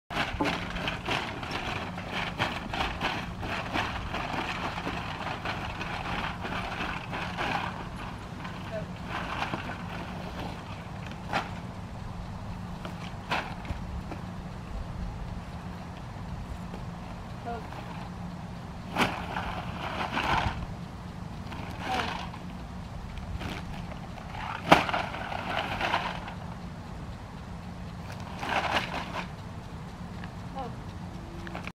Okan Empress
This is a short video of Okan Empress and her reaction to the bag of cans.